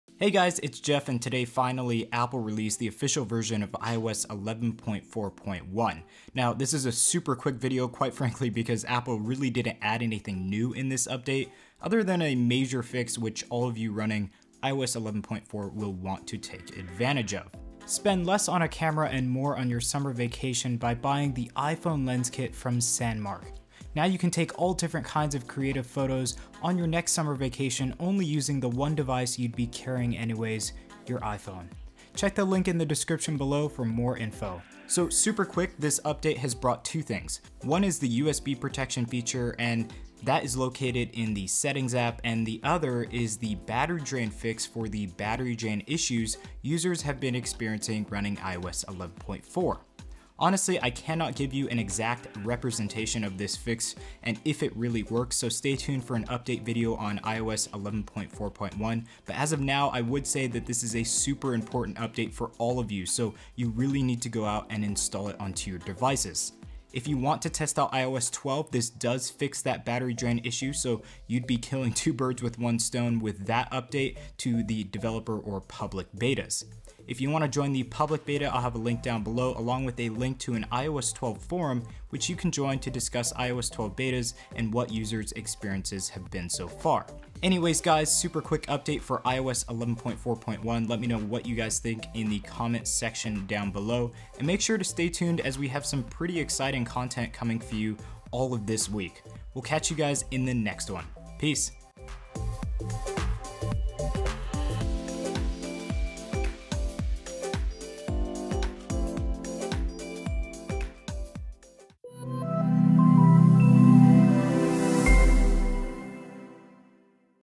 iOS 11.4.1 Released! Battery Drain Fix & More!
Apple released iOS 11.4.1 today after a very lengthy Beta period. This update is long overdue, so let’s take a look at the changes Apple made in this update and why you might want to be installing it onto your device.

Check out all of the gear that we use!